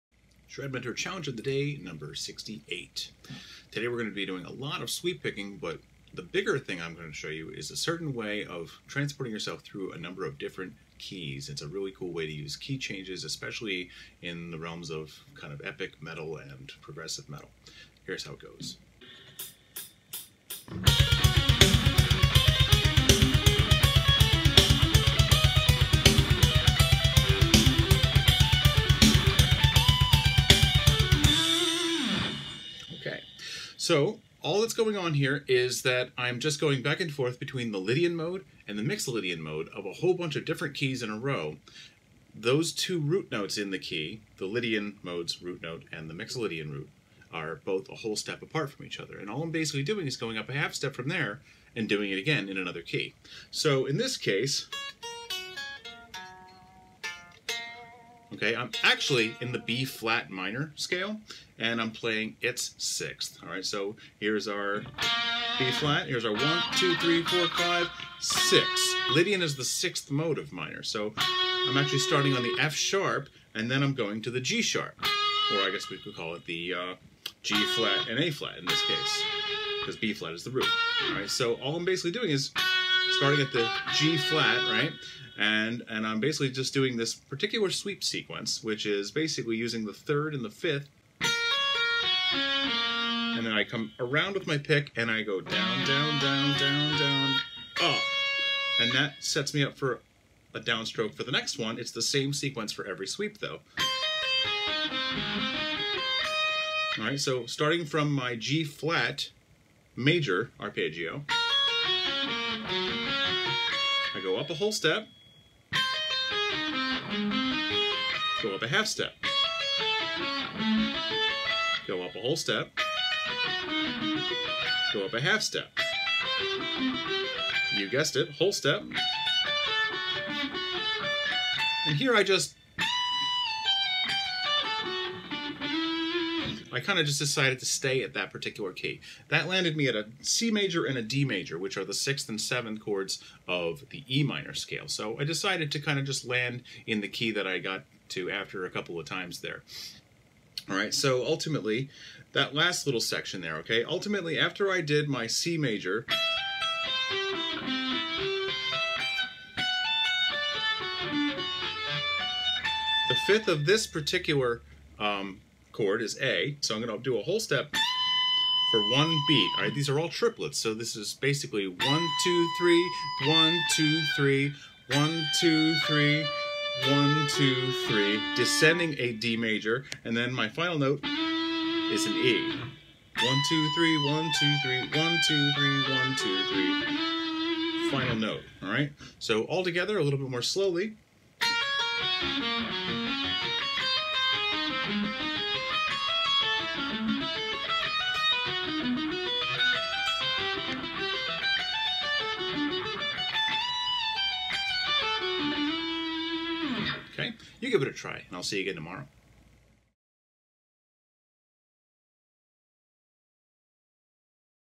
ShredMentor Challenge of the Day #68
Get your sweep picking chops ready - today's challenge features a handy and easy to remember key-changing progression using the Lydian and Mixolydian modes.